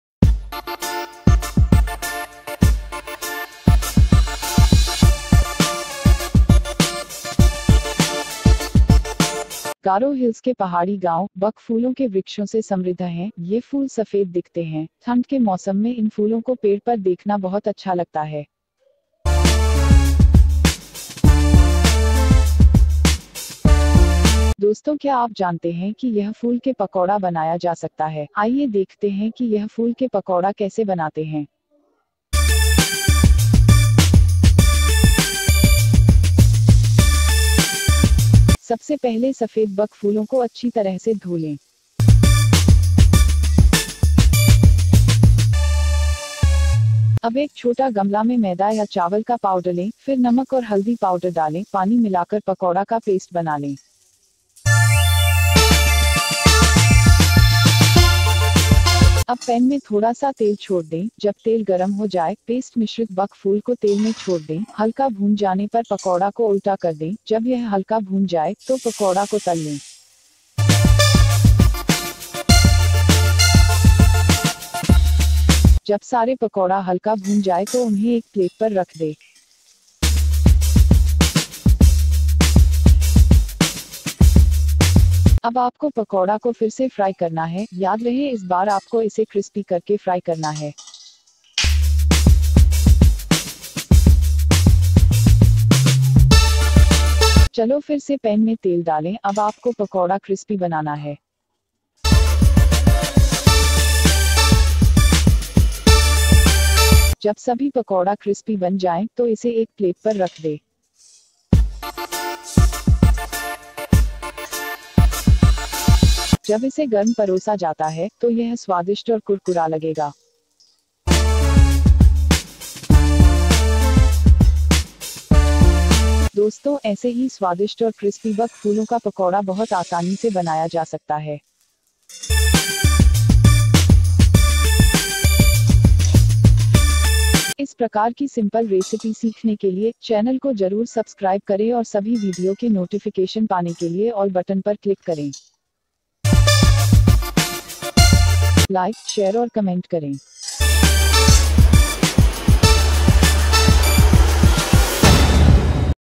Crispy Bak Fuler Pakora Simple Style, বক ফুলের বড়া, बक फूलों का पकौड़ा
गारो हिल्स के पहाड़ी गाँव, बक फूलों के वृक्षों से समृद्ध हैं। ये फूल सफेद दिखते हैं। ठंड के मौसम में इन फूलों को पेड़ पर देखना बहुत अच्छा लगता है।
दोस्तों क्या आप जानते हैं कि यह फूल के पकोड़ा बनाया जा सकता है।आइए देखते हैं कि यह फूल के पकोड़ा कैसे बनाते हैं ।सबसे पहले सफेद बक फूलों को अच्छी तरह से धो लें।अब एक छोटा गमला में मैदा या चावल का पाउडर लें।फिर नमक और हल्दी पाउडर डालें। पानी मिलाकर पकोड़ा का पेस्ट बना लें।अब पैन में थोड़ा सा तेल छोड़ दें। जब तेल गरम हो जाए, पेस्ट मिश्रित बक फूल को तेल में छोड़ दें। हल्का भून जाने पर पकोड़ा को उल्टा कर दें। जब यह हल्का भून जाए, तो पकौड़ा को तल लें।जब सारे पकौड़ा हल्का भून जाए तो उन्हें एक प्लेट पर रख दे।अब आपको पकौड़ा को फिर से फ्राई करना है। याद रहे इस बार आपको इसे Crispy करके फ्राई करना है।चलो फिर से पैन में तेल डालें। अब आपको पकौड़ा क्रिस्पी बनाना है।जब सभी पकौड़ा Crispy बन जाएं, तो इसे एक प्लेट पर रख दे।जब इसे गर्म परोसा जाता है, तो यह स्वादिष्ट और कुरकुरा लगेगा। दोस्तों, ऐसे ही स्वादिष्ट और Crispy बक फूलों का पकौड़ा बहुत आसानी से बनाया जा सकता है।